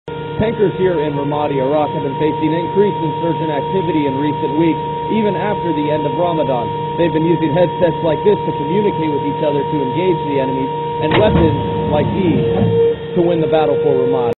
Every tank loader must have one of these! CRITICAL TANK LOADING
Without this key piece of military hardware, you are done for as a tank loader. Some might even say Rircardo'd.

Sorry I was just having fun lol.

Main Battle Tanks need gun ammunition like all other weapons of war. The tank is only as good as the loader that is providing the firepower. The main guns of most modern day tanks require the shells to be loaded either manually by a tank crewman or automatically by a tank auto-loader. The autoloader has flaws as does any tank soldier. And example being the T-90 and the M1A2 Abrams, the vehicles have their own way of fighting on the battlefield.

Attire For Effect💣 💥 📸 Also follow them on Instagram: # attire_for_effect